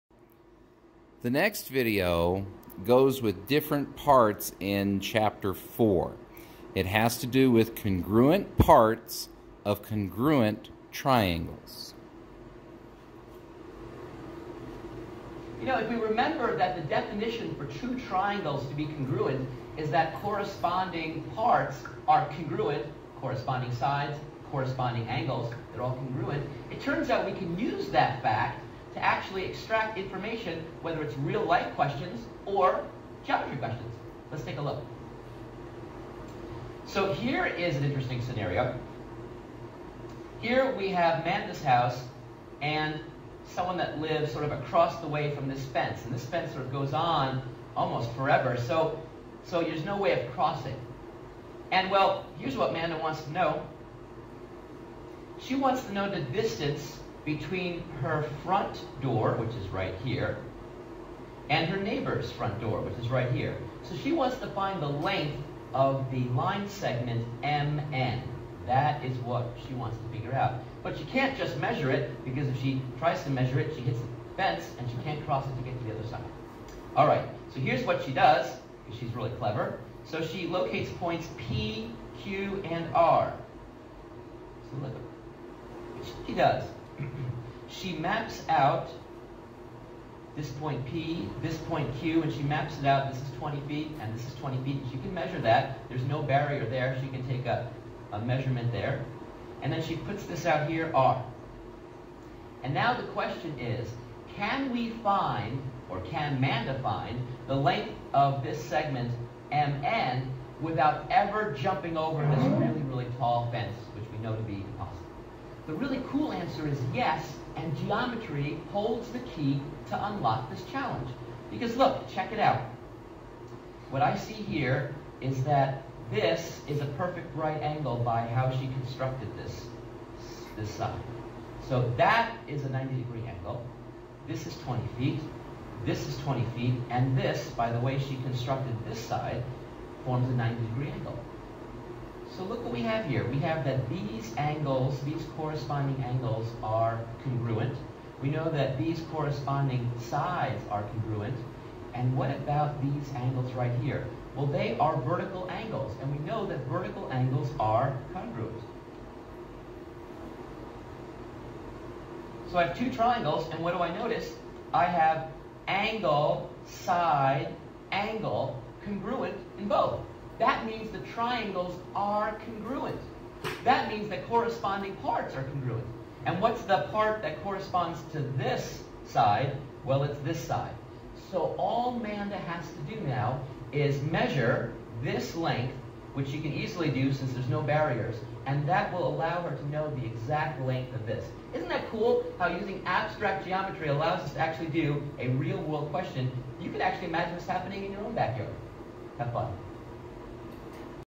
Ch 4, Congruent parts of Congruent Triangles
Professor Burger explains Congruent parts of a Congruent Triangle.  This video covers various locations with in the text for Topic 4.

From Holt Geometry Lesson Tutorial Videos, Holt, Rinehart and Winston, approximately 2007.